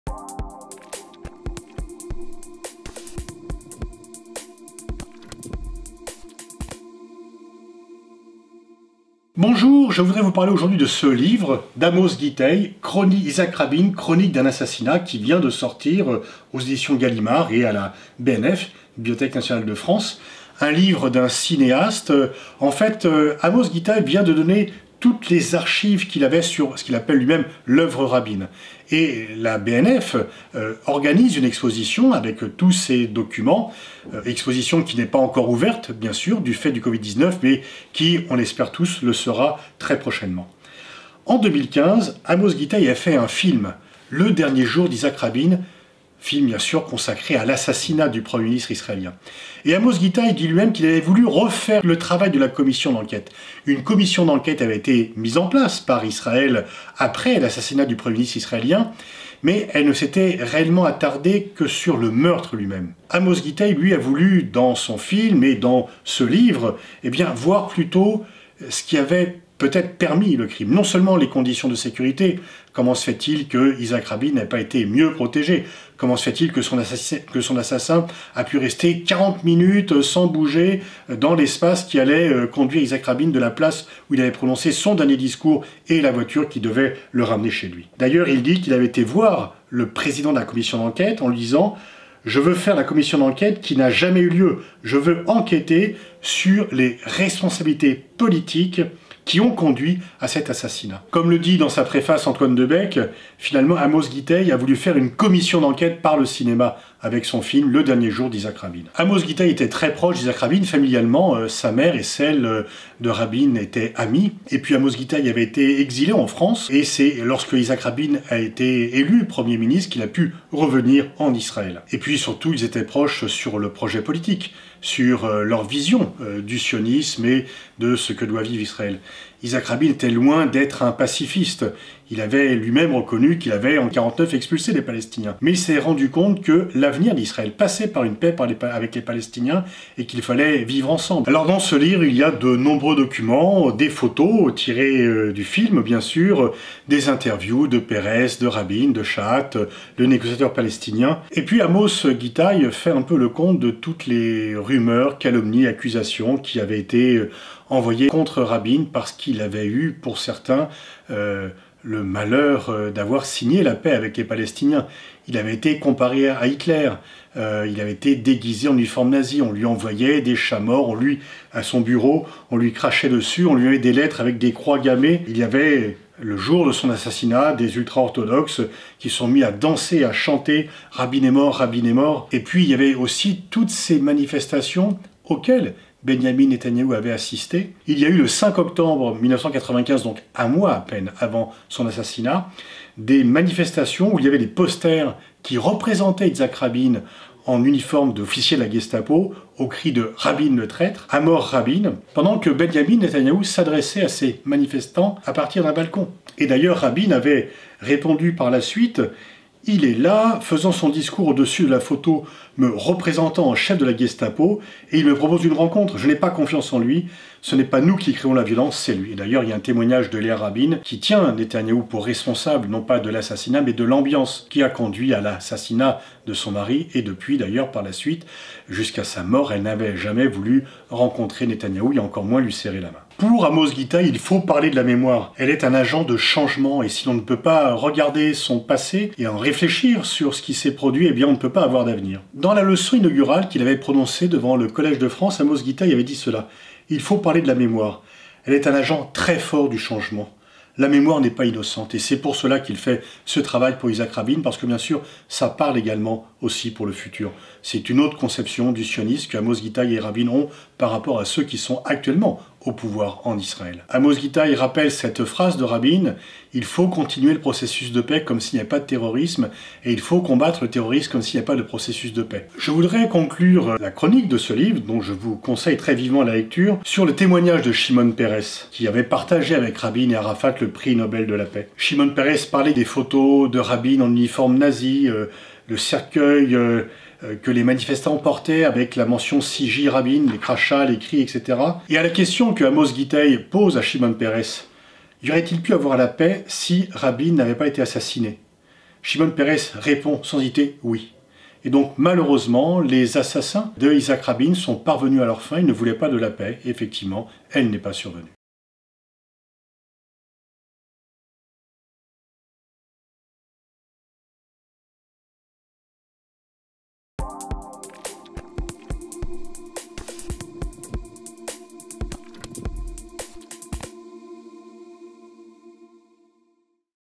J'ai lu... "Yitzhak Rabin : Chroniques d'un assassinat" d'Amos Gitaï
Le cinéaste israélien Amos Gitaï concentre dans cet ouvrage l'ensemble des archives qu'il a collectées sur ce qu'il appelle lui même "l'oeuvre Rabin", archives qui feront également l'objet d'une exposition à la Bibliothèque nationale de France (BNF). Ce superbe ouvrage, édité par Gallimard et la BNF, est l'occasion de revenir sur l'œuvre politique de Rabin, seul dirigeant israélien qui aura permis d'entrevoir la paix au Proche-Orient, avant qu'elle ne s'éloigne à nouveau.